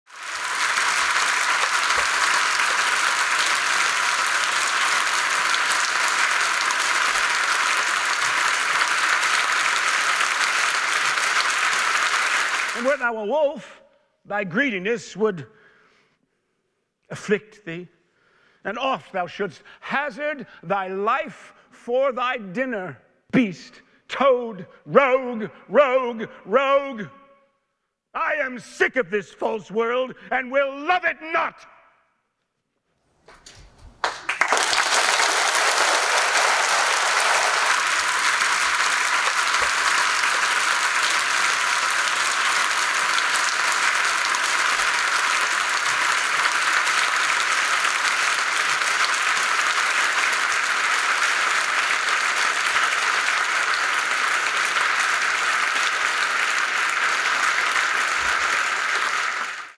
Kevin Spacey receives standing ovation at Oxford lecture on cancel culture
Kevin Spacey was given a standing ovation by an audience at the University of Oxford last night (16 October), after he delivered a performance as part of a lecture about “cancel culture”.

It was the first time the House of Cards actor, 64, has performed on stage since the US star was acquitted of sexual assault against four men in a London trial that ended in July 2023.

Spacey delivered a five-minute scene from Timon of Athens, William Shakespeare’s satire of wealth, greed and betrayal. Modern critics have described the work as an exploration of what is now often called “cancel culture”.